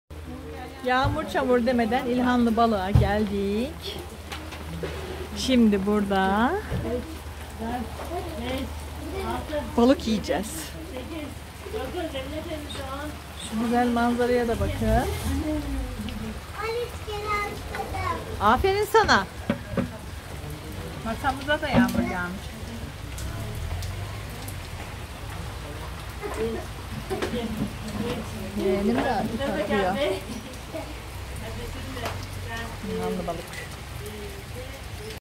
ANKARADA LEZZETLİ BALIK. VLOG
Ankarada gidip gezebileceğiniz güzel bir ortam.
Etimesguta 40 km kadar. ilhanlı balık. güdül yolunda.